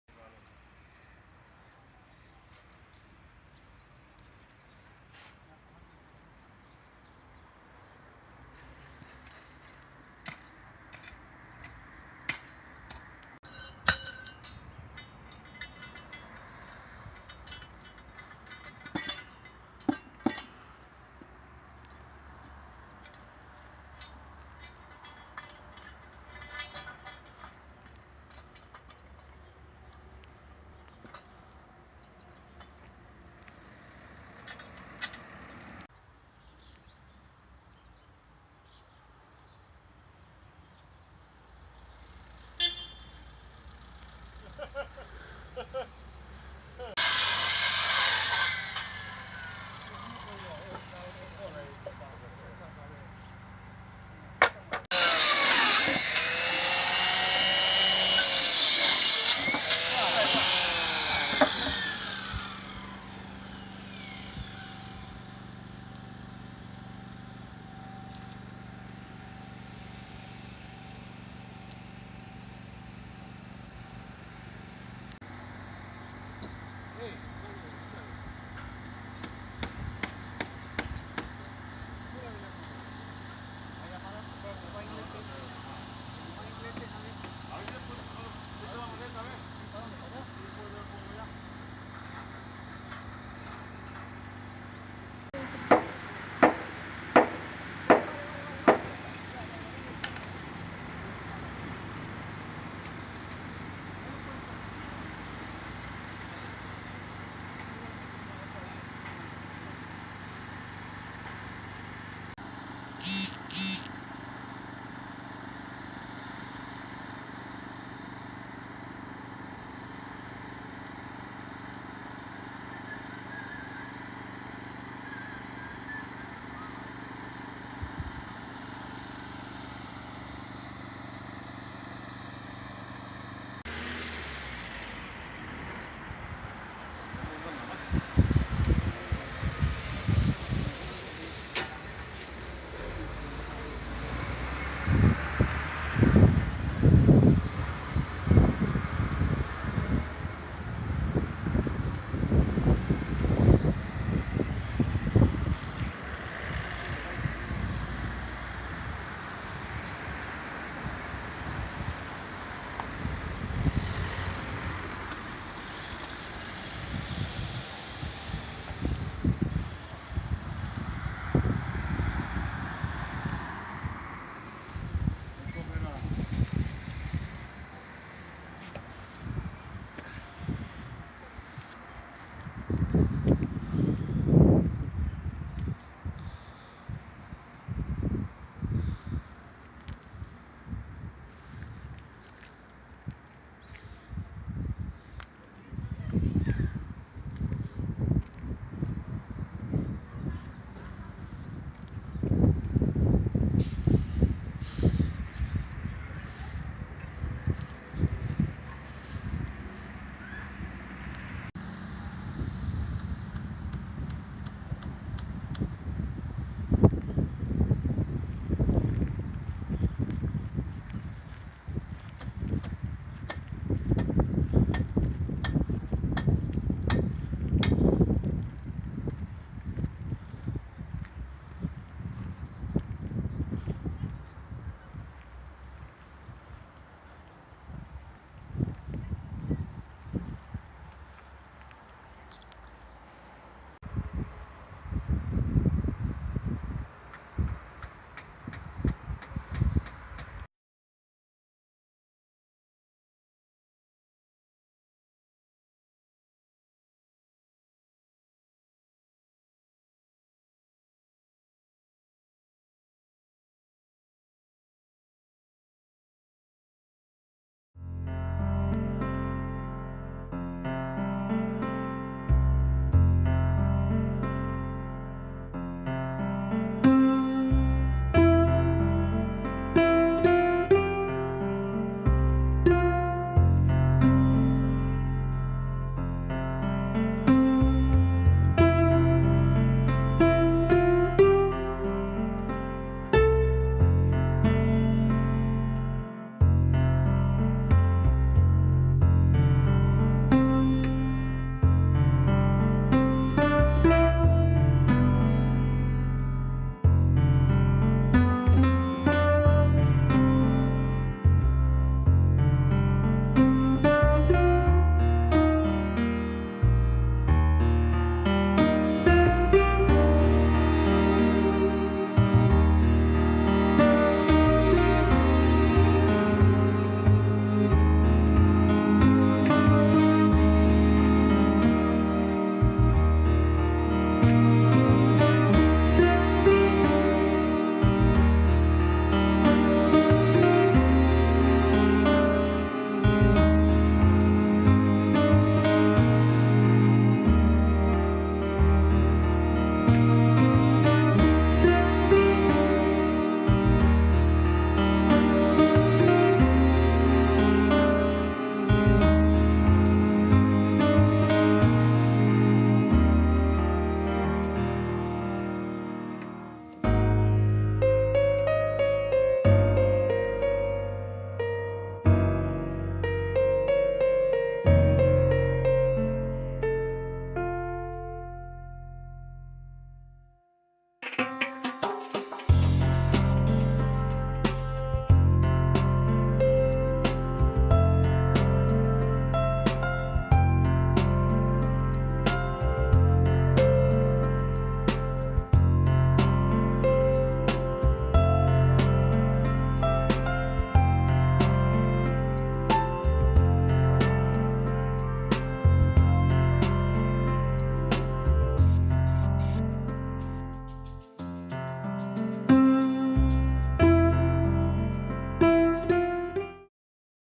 Apuntalamiento de la Ermita del Loreto. Tembleque.
A finales de febrero de 2013, ante el riesgo de derrumbe.